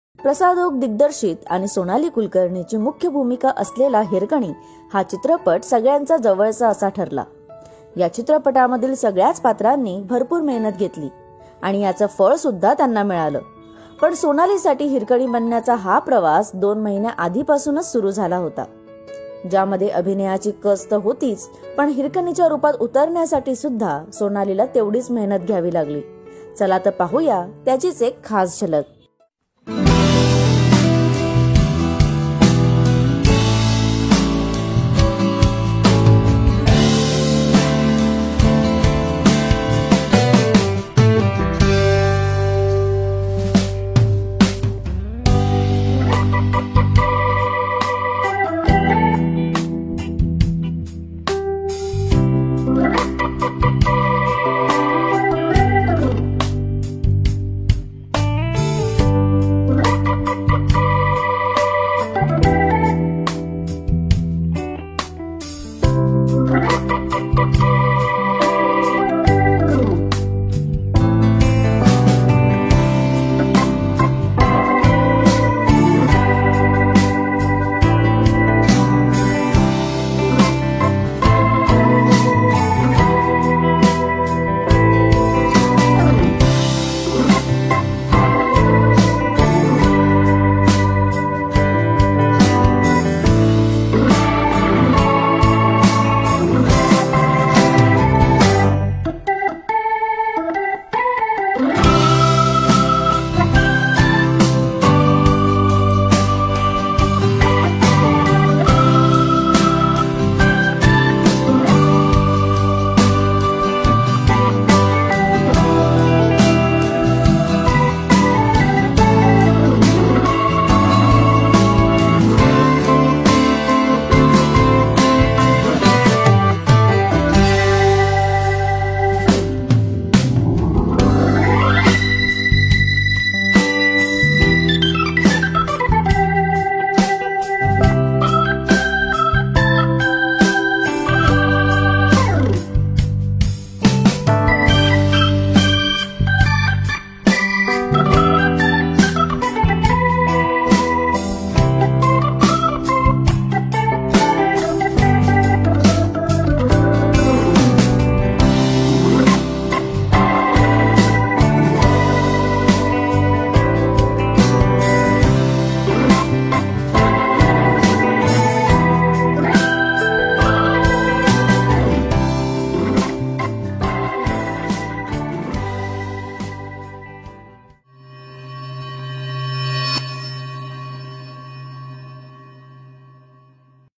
Sonalee Kulkarni | भूमिकेसाठी नाक टोचलं तेव्हा | Hirkani | Prasad Oak | Itsmajja
अभिनेत्री सोनाली कुलकर्णी हिने हिरकणीच्या रूपात रंगण्यासाठी कशी मेहनत घेतली, चला पाहूया एक झलक...